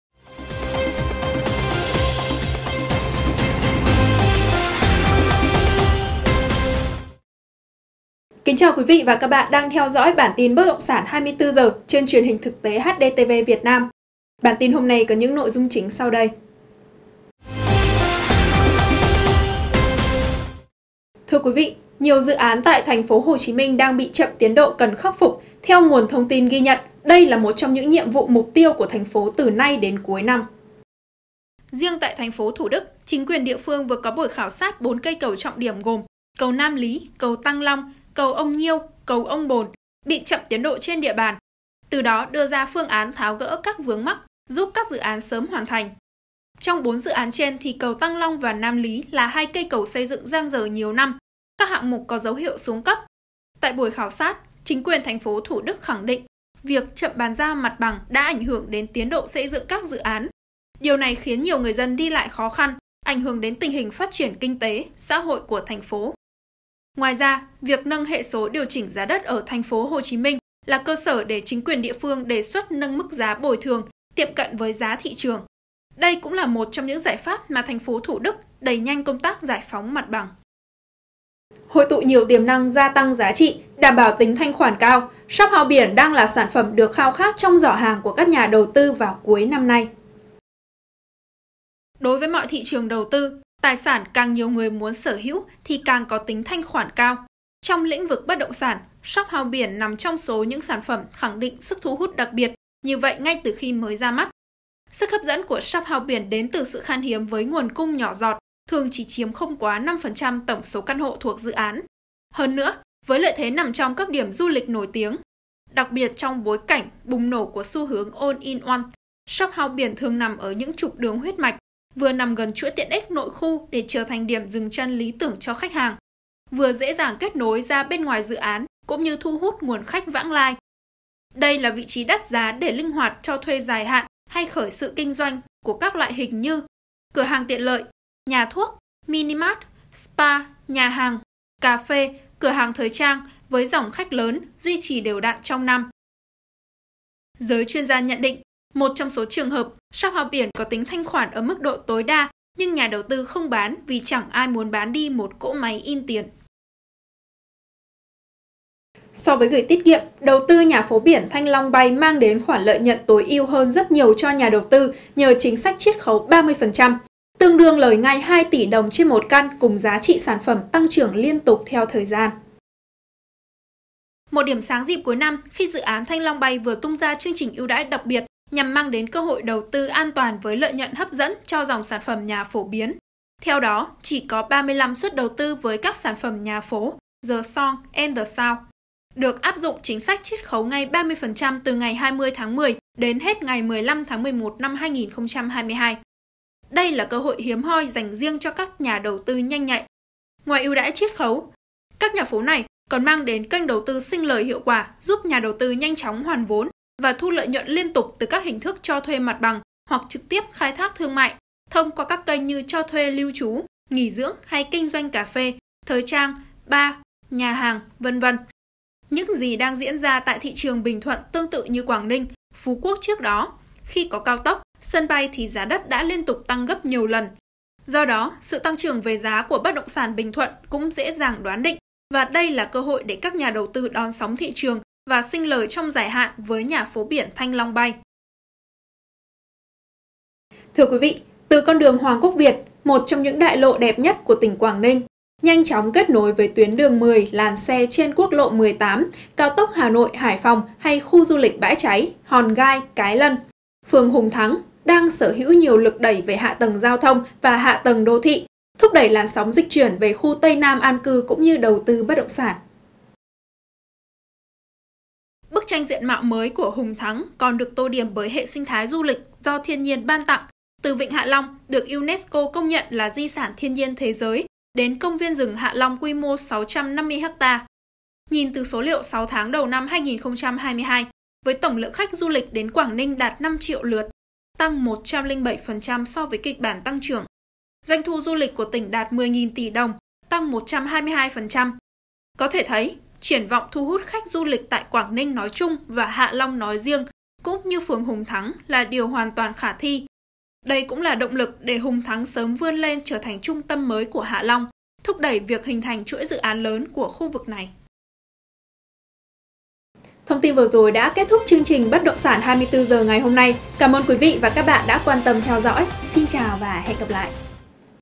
BẢN TIN BẤT ĐỘNG SẢN | SỐ 6
CÓ CÁC NỘI DUNG SAU ĐÂY:

1. TP HỒ CHÍ MINH NHIỀU DỰ ÁN CHẬM TIẾN ĐỘ .

2. SỨC HÚT TỪ SHOPHOUSE BIỂN: THANH KHOẢN CAO, NHIỀU TIỀM NĂNG TĂNG GIÁ.

3. NHÀ PHỐ BIỂN THANH LONG BAY: SINH LỜI NGAY VỚI CHÍNH SÁCH CHIẾT KHẤU 30%.

4. ĐỘNG LỰC NÀO GIÚP HÙNG THẮNG TRỞ THÀNH TRUNG TÂM MỚI PHÍA TÂY HẠ LONG?